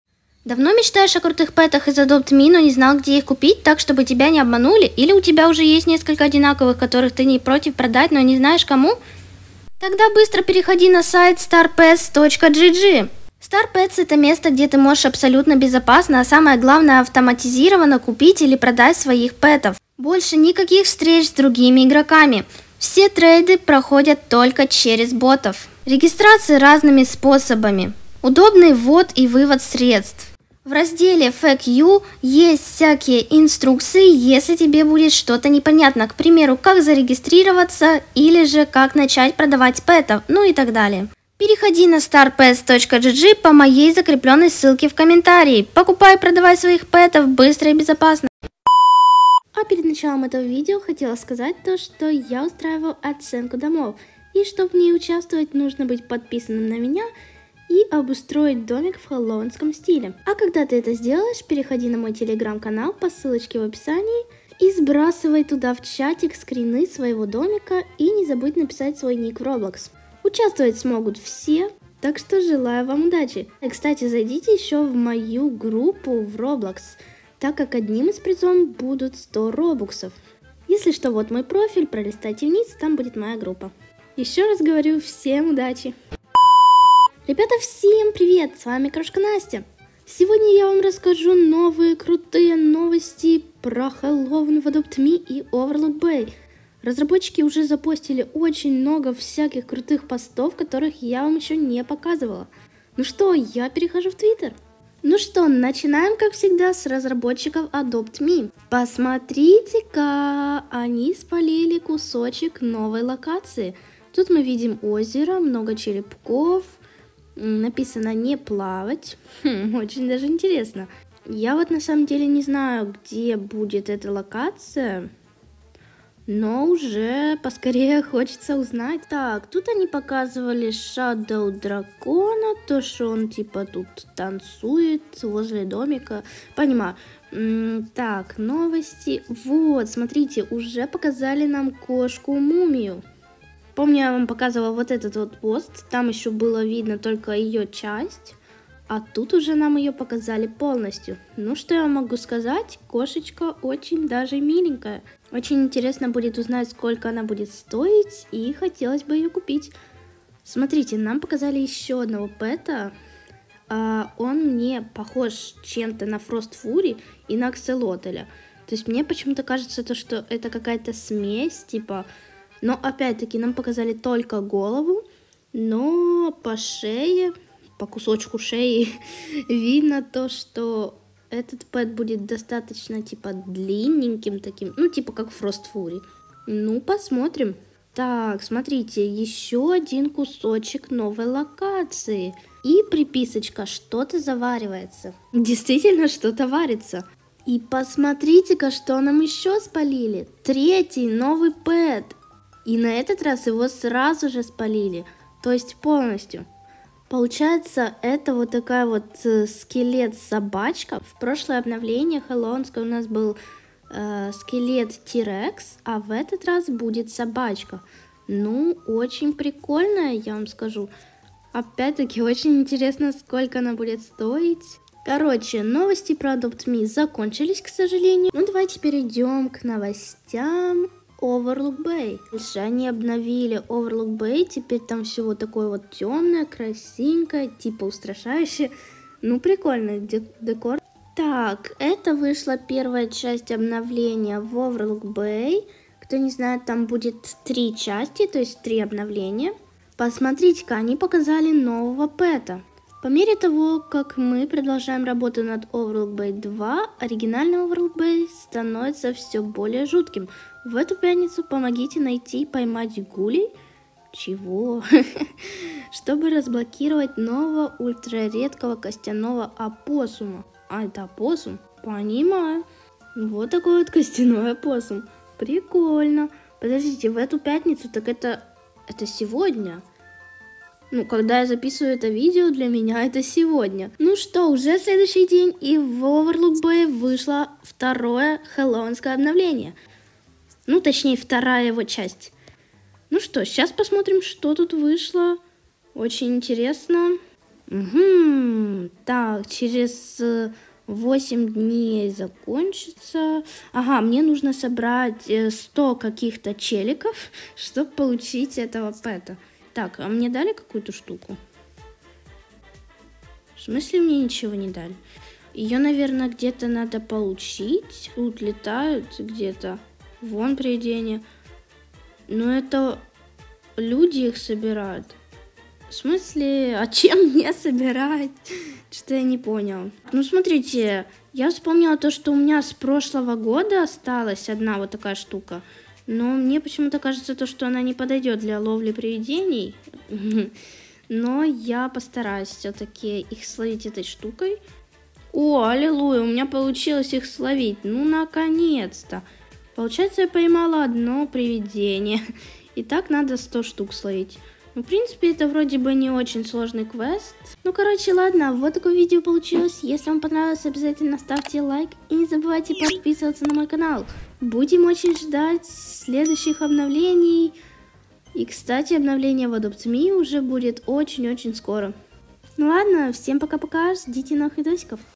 УЖЕ ПОКАЗАЛИ 3 НОВЫХ ПЕТА?! ЧТО БУДЕТ НА ХЕЛЛОУИН В АДОПТ МИ?! НОВОСТИ ADOPT ME Kroshka_Nastia
Всем приветик✌️ За последнюю неделю разработкичи адопт ми и оверлук бей выложили много всего интересного в своем твиттер! И в этом видео я решила вам показать все новости! Скорее смотри видео, если тебе интересно какие у нас будут петы и что ещё показали разработчики😉

Кстати я решила устроить оценку домов! Для участия нужно обустроить дом в хеллоуинском стиле, все смогут поучаствовать! Слушай внимательно начало видео, чтоб узнать подробнее😉